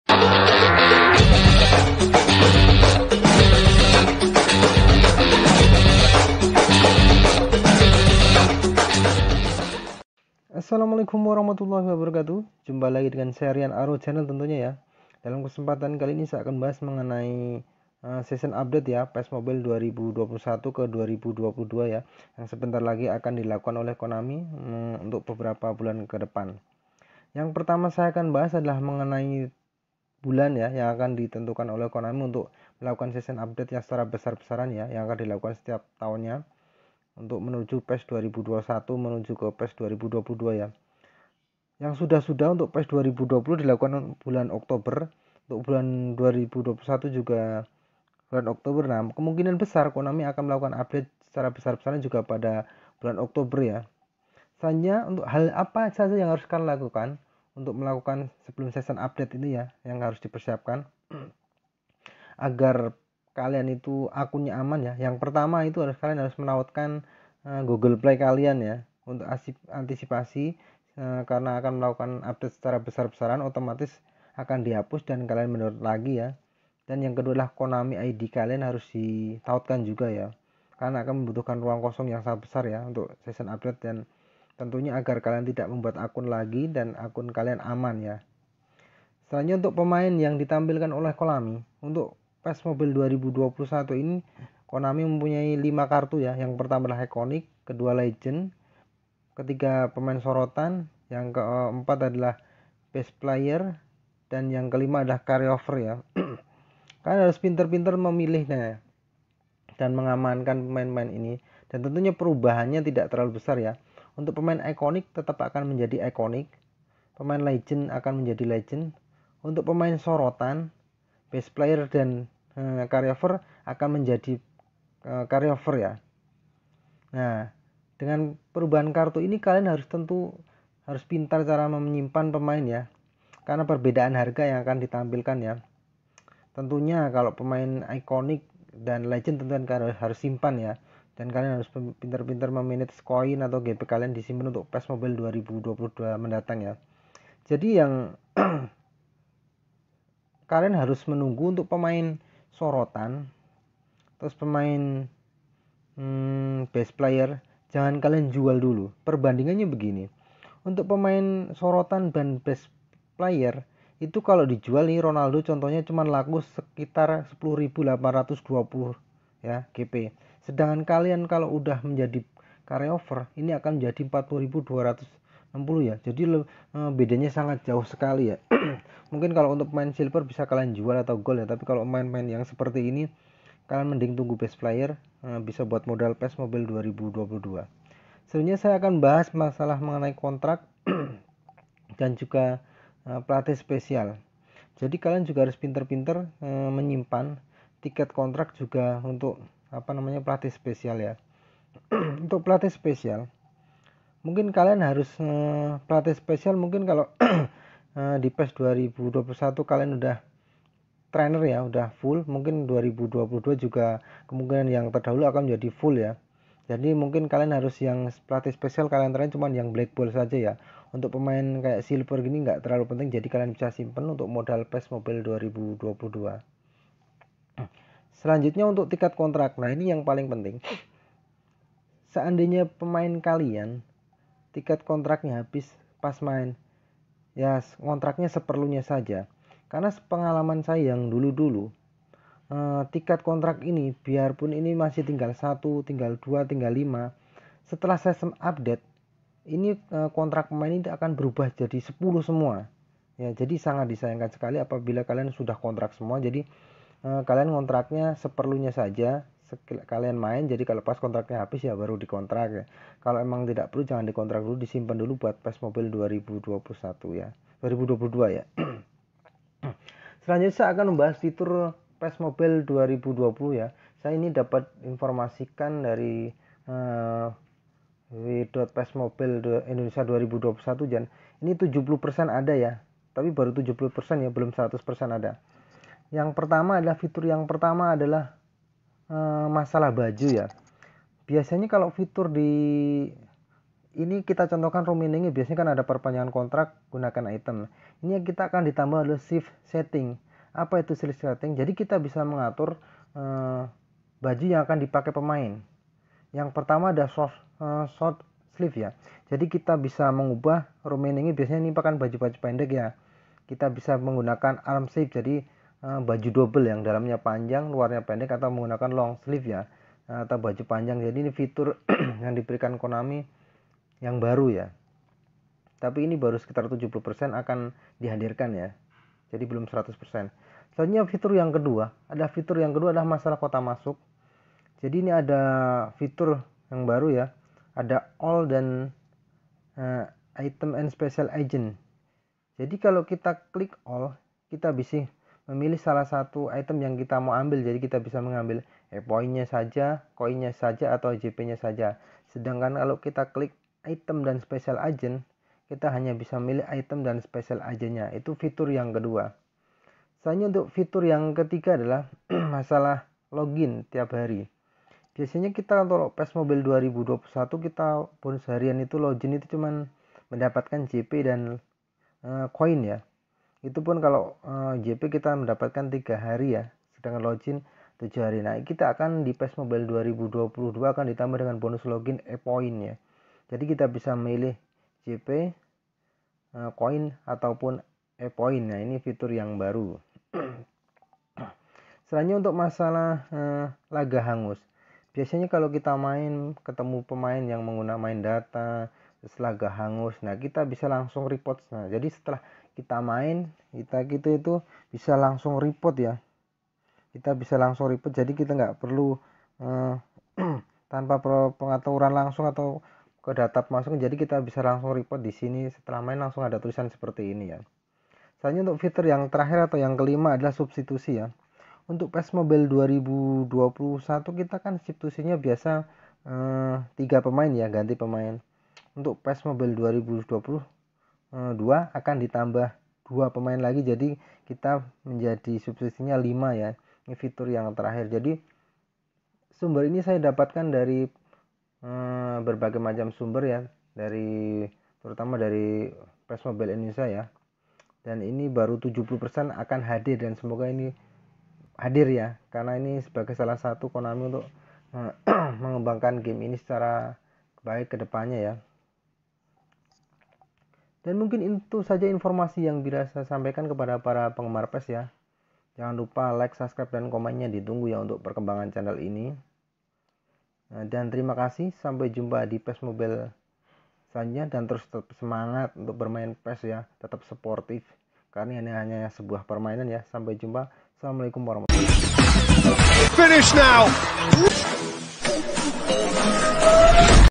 PENJELASAN LENGKAP UPDATE PES 2022 FITUR LEBIH KEREN
Berikut adalah penjelasan mengenai update yang akan dilakukan oleh Konami dari PES 2021 menuju PES 2022.untuk informasi selengkapnya tonton video berikut ini.
pes mobile
pes update 2022#pes22#newplayer#uogrades#pesratings#ratingrefresh#news#pes2021#fifa2021#efootball#efootballpes